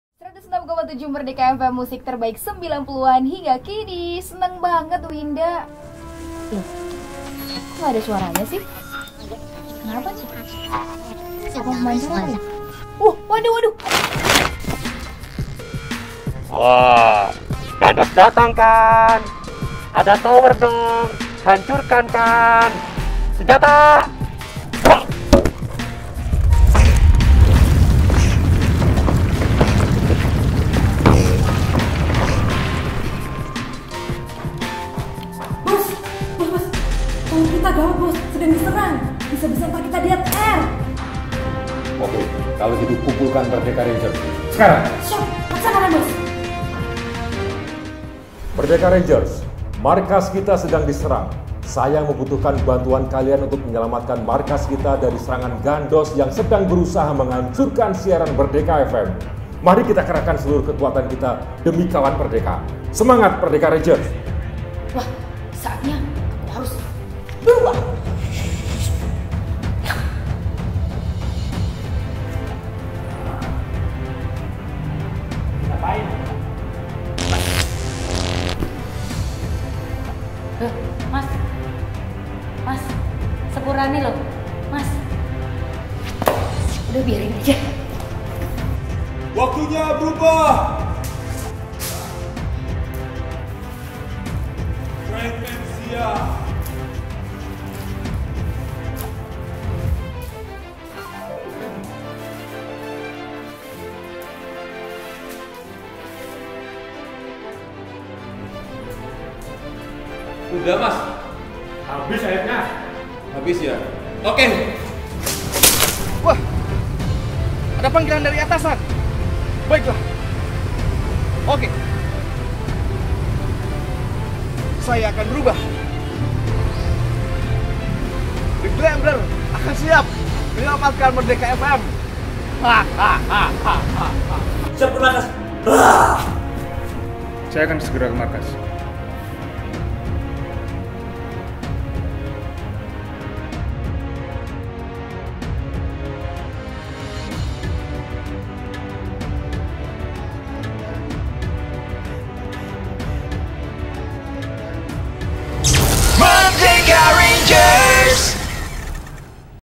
Merdeka Rangers - "Saving Merdeka"
Gawat, station Merdeka FM sedang diserang oleh Gandos !
Ia tak ingin Kawan Merdeka mendengarkan Merdeka FM lagi. Tapi para superhero Merdeka FM tidak tinggal diam untuk mengamankan markasnya demi Kawan Merdeka.

Mereka adalah MERDEKA RANGERS !